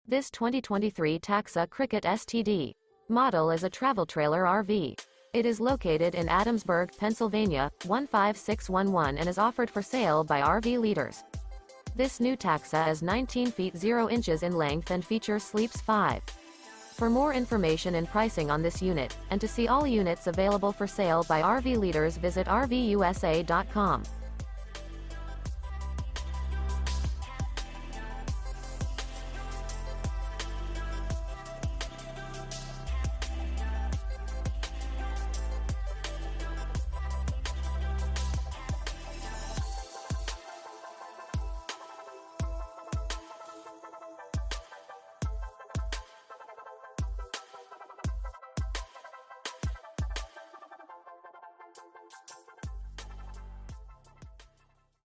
Incredible 2023 Taxa Cricket Travel Trailer RV For Sale in Adamsburg, PA | RVUSA.com
This 2023 Taxa Cricket Std. Model is a Travel Trailer RV. It is located in Adamsburg, PA 15611 and is offered for sale by RV Leaders . This New Taxa Is 19' 0" in length and features sleeps 5..